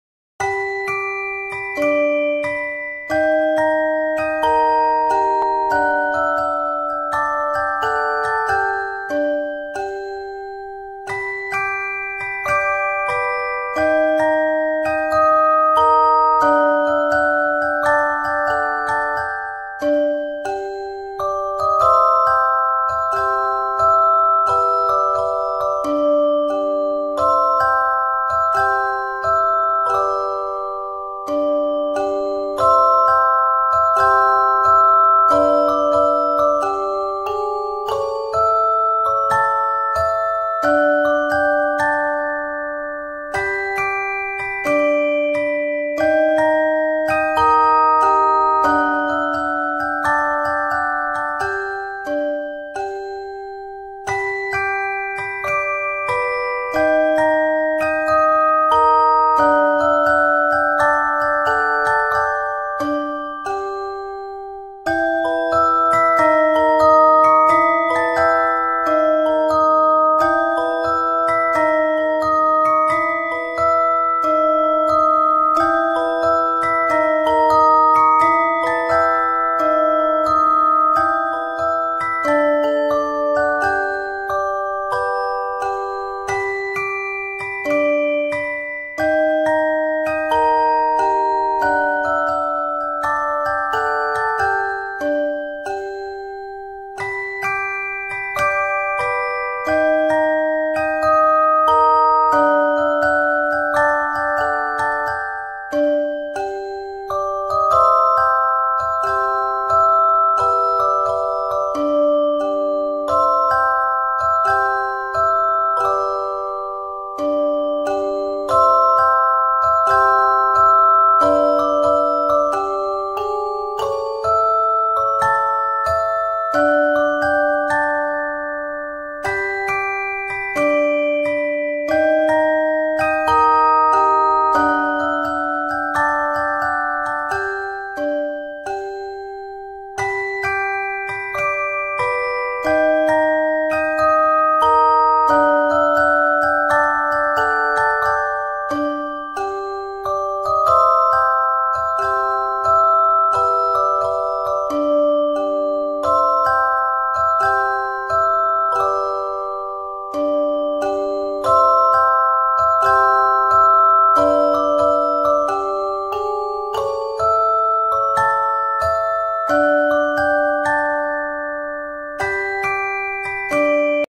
House Colouring and Drawing for kids, How to draw a House step by step,2019
In this video I show you House Colouring and Drawing for kids, How to draw a House step by step,2019, Artistica Min